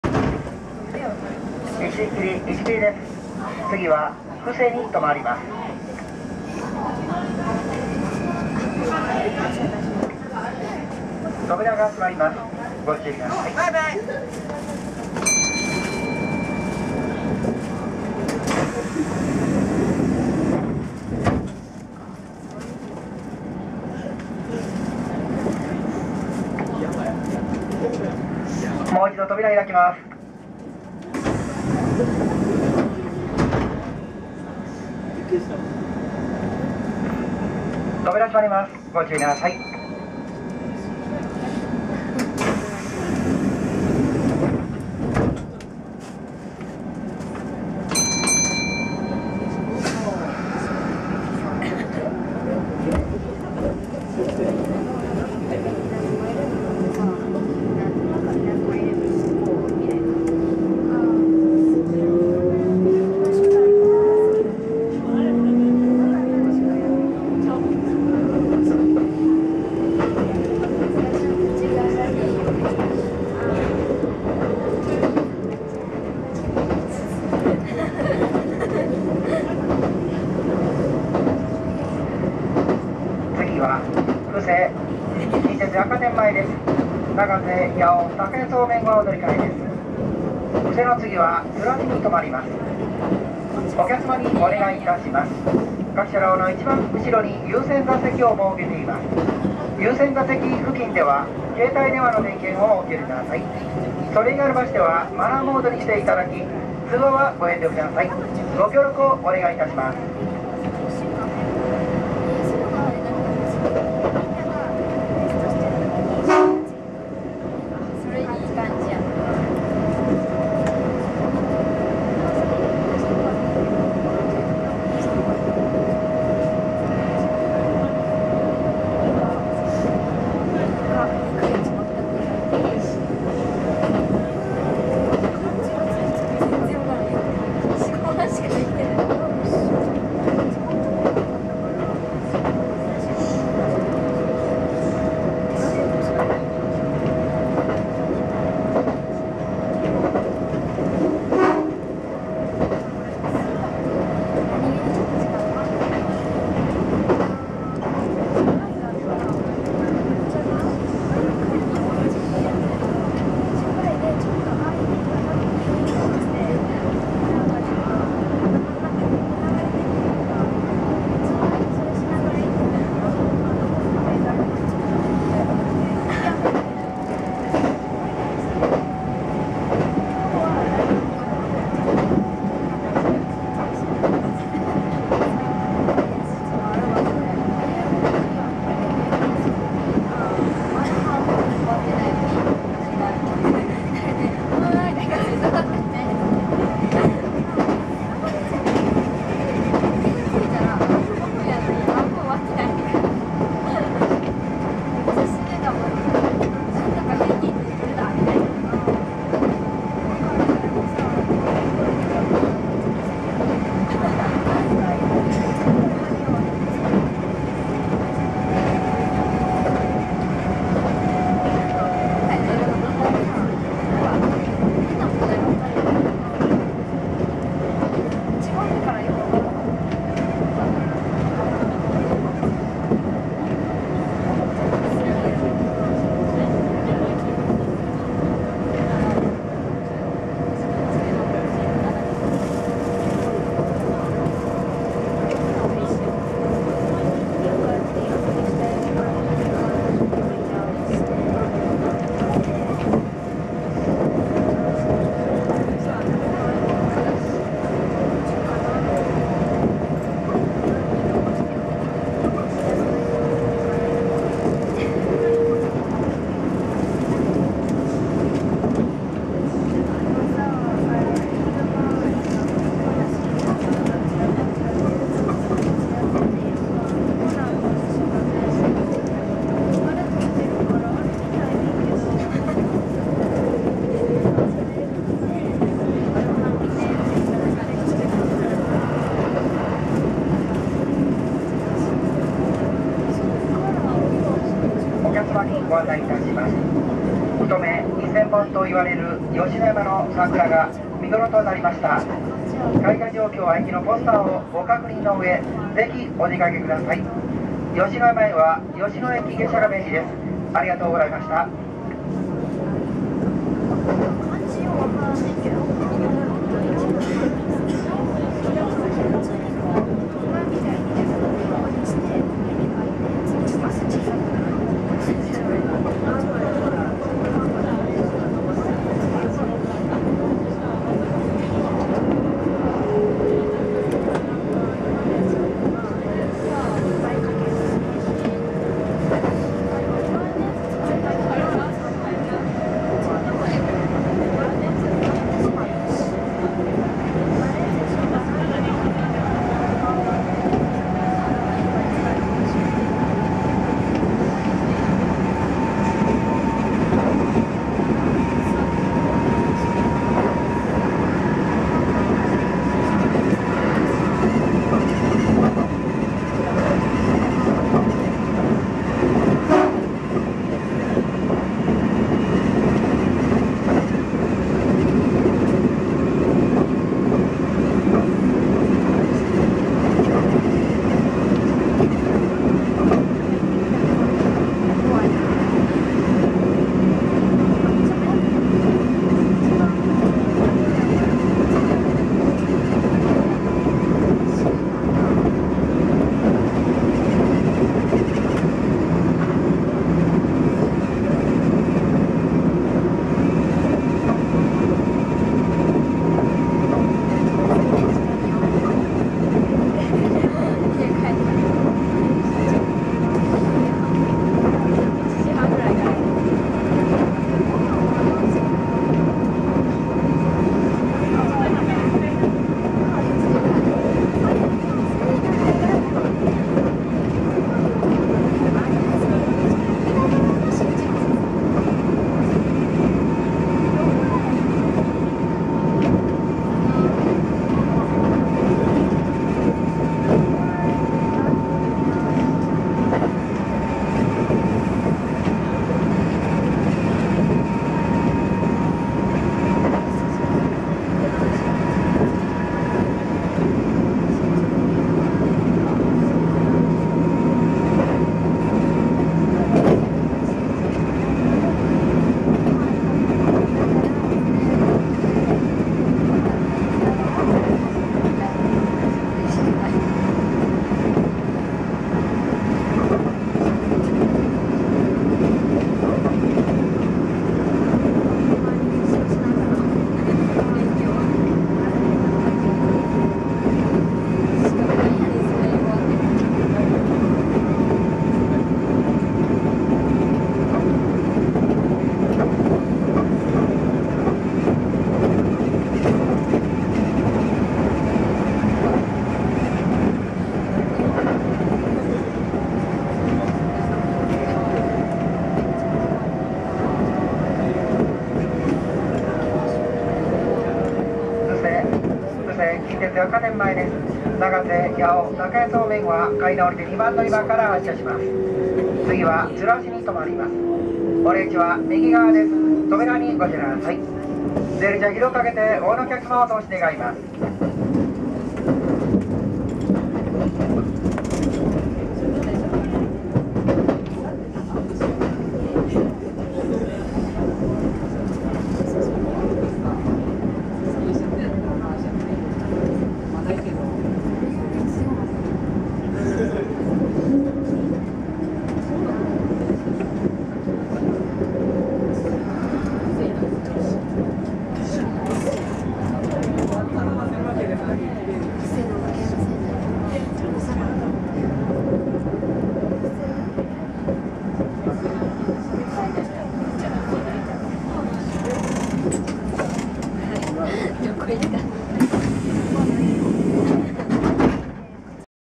近鉄奈良線急行 1233系 石切～布施 前面展望
1233系急行。大阪難波行。